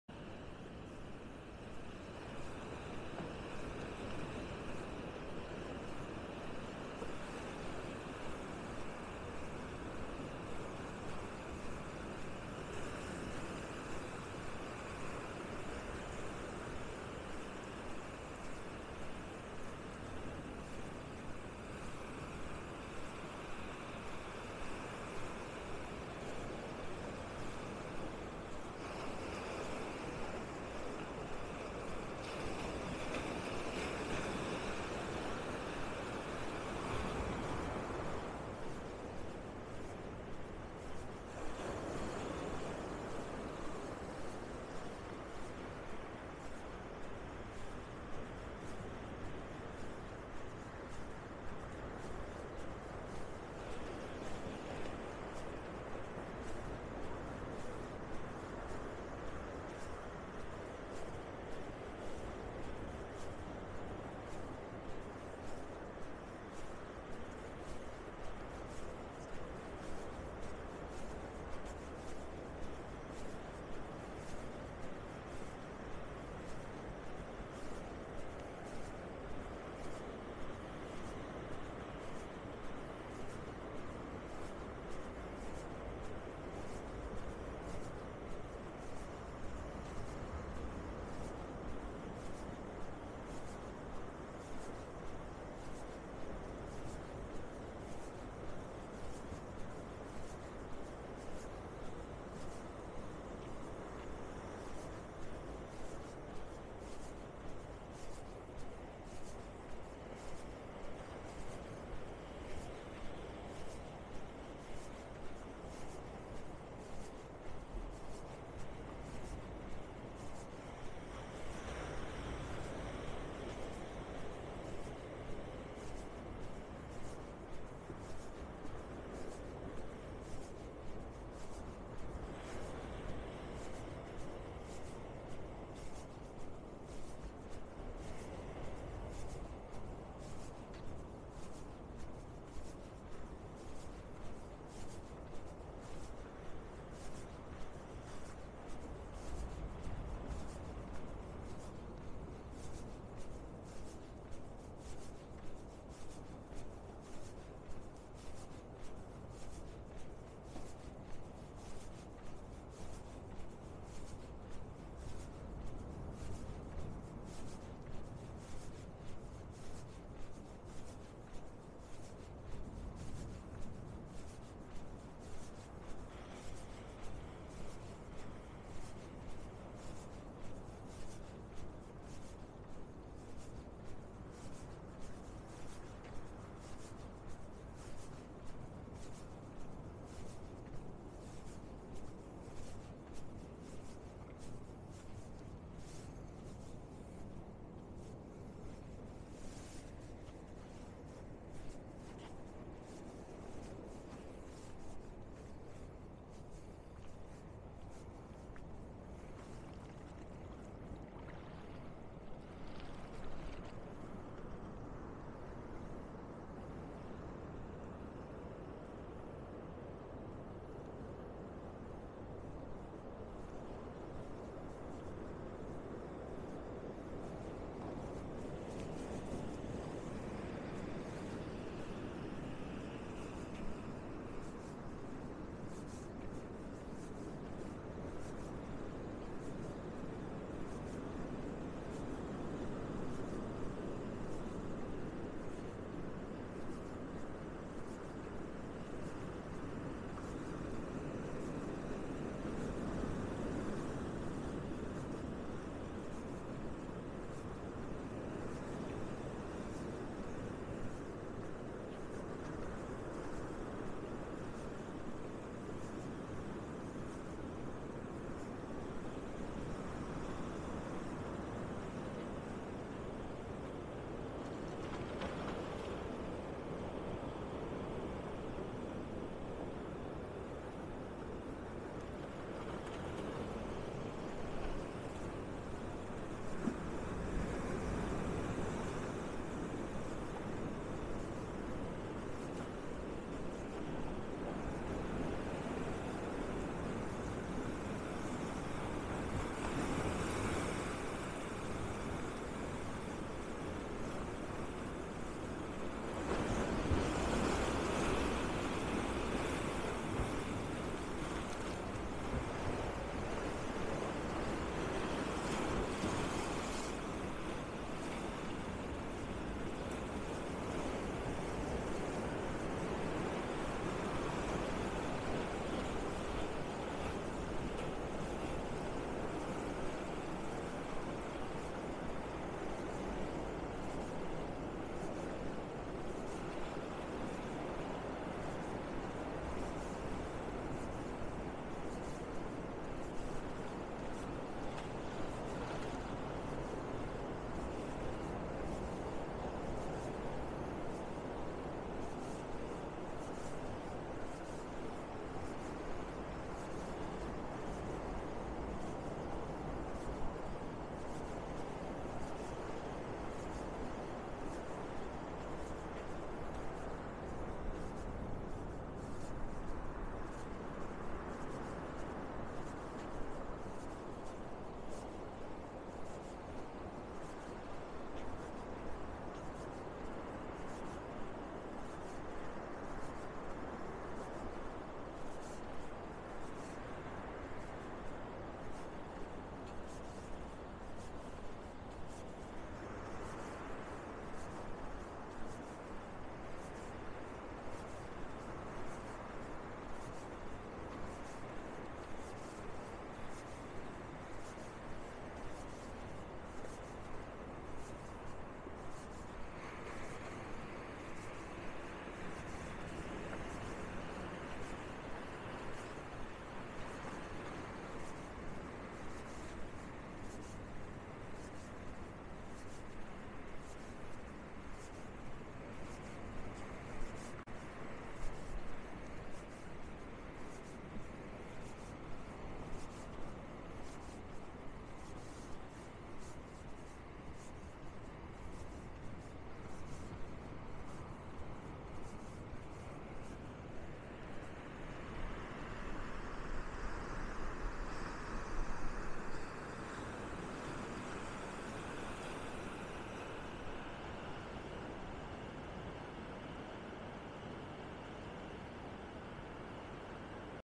En tur på Løkken mole i mørket fredag d. 9. oktober 2021
Kom med en tur på Løkken mole i aftenmørket med lyden af det næsten usynlige hav.